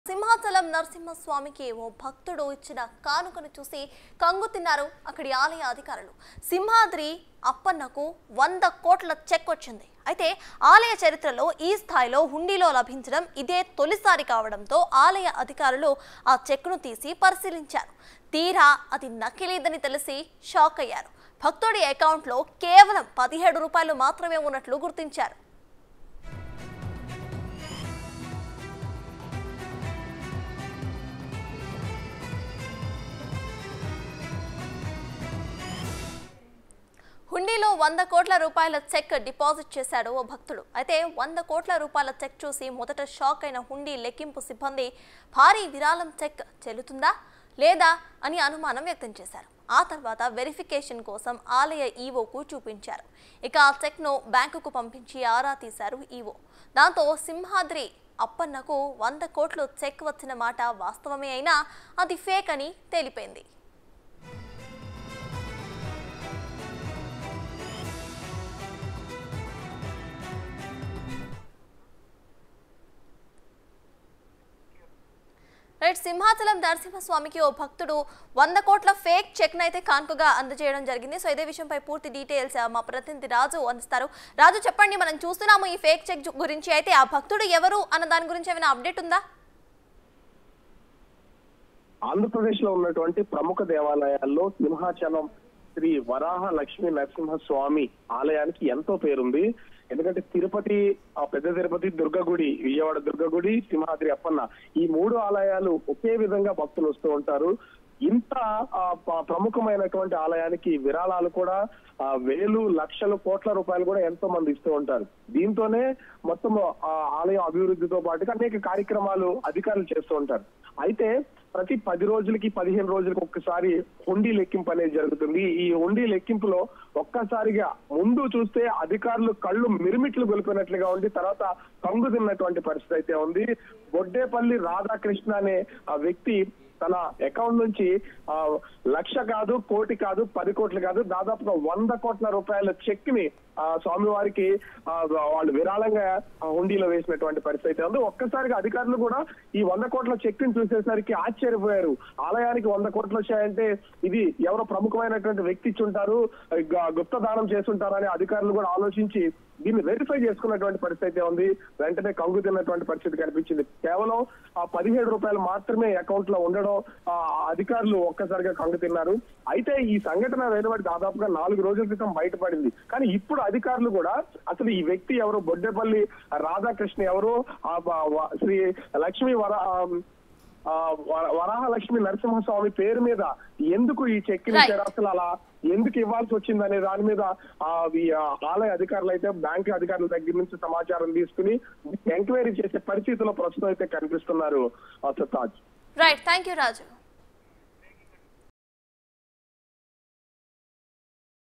100 Crore Cheque In Hundi : దేవుని హుండీలో 100 కోట్ల చెక్.. ట్విస్ట్ చూస్తే మైండ్ బ్లాక్ | BIG TV
సింహాచలం నరసింహ స్వామికి ఓ భక్తుడు ఇచ్చిన కానుకను చూసి కంగుతిన్నారు అక్కడి ఆలయ అధికారులు.  సింహాద్రి అప్పన్నకు రూ.100 కోట్ల చెక్‌ వచ్చింది.అయితే, ఆలయ చరిత్రలో ఈ స్థాయిలో హుండీలో లభించడం ఇదే తొలిసారి కావడంతో ఆలయ అధికారులు ఆ చెక్‌ను తీసి పరిశీలించారు. తీరా అది నకిలీదని తెలిసి షాక్ అయ్యారు. భక్తుడి అకౌంట్లో కేవలం 17 రూపాయలు ఉన్నట్టు గుర్తించారు.
హుండీలో 100 కోట్ల రూపాయల చెక్ డిపాజిట్ చేశారు ఓ భక్తుడు. అయితే 100 కోట్ల రూపాయల చెక్ చూసి మొదట షాక్ అయిన హుండీ లెక్కింపు సిబ్బంది.. భారీ విరాళం చెక్ చెల్లుతుందా..? లేదా..? అని అనుమానం వ్యక్తం చేశారు.ఆ తర్వాత వెరిఫికేషన్‌ కోసం ఆలయ ఈవోకు చూపించారు.. ఇక,ఆ చెక్‌ను బ్యాంకుకు పంపించి ఆరా తీశారు ఈవో.. దాంతో, సింహాద్రి అప్పన్నకు వందకోట్లు చెక్ వచ్చిన మాట వాస్తవమే అయినా.. అది ఫేక్‌ అని తేలిపోయింది.

భక్తుడిచ్చిన షాక్ కి.. పూజారుల మైండ్ బ్లాక్